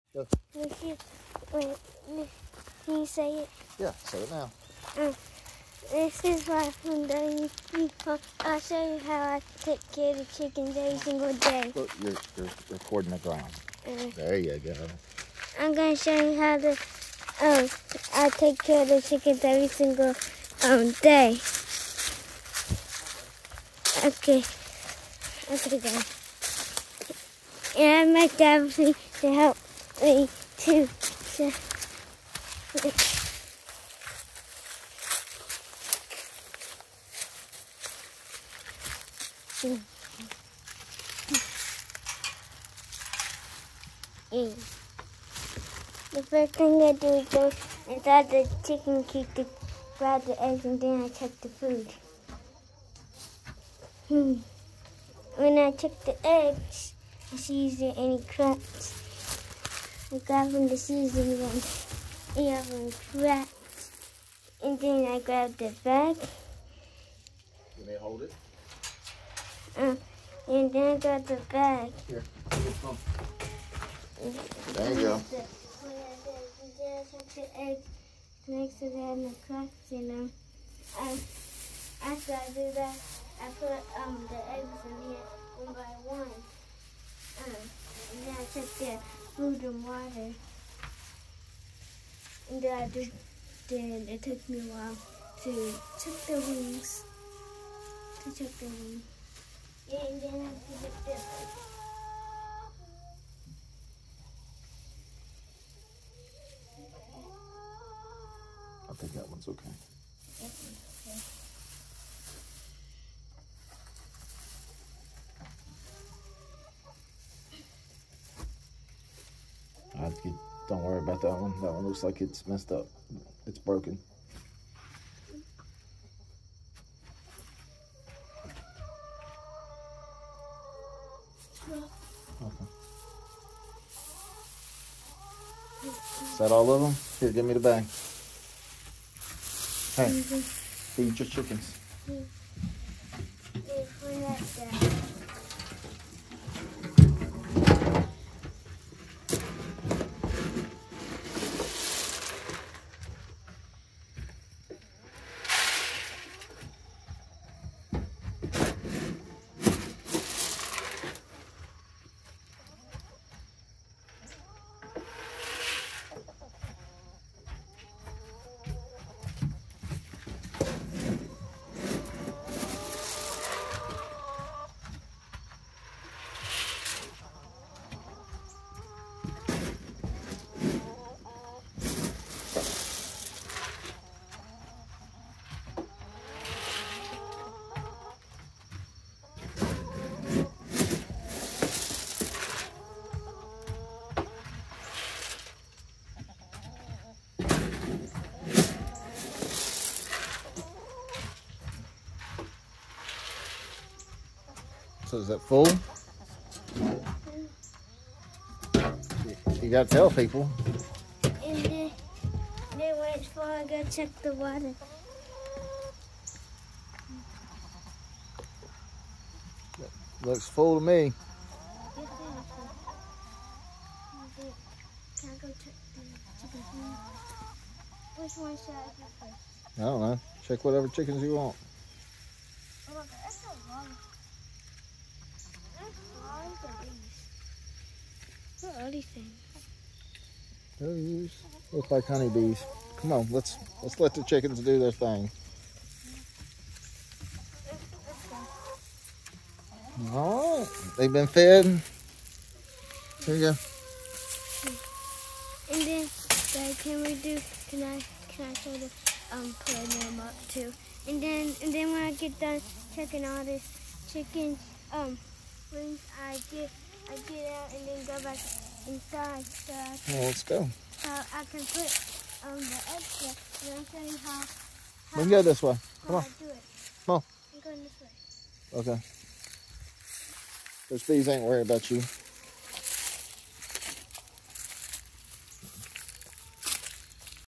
Wyatt and Chickens
Wyatt wanted to show how he takes care of his chickens.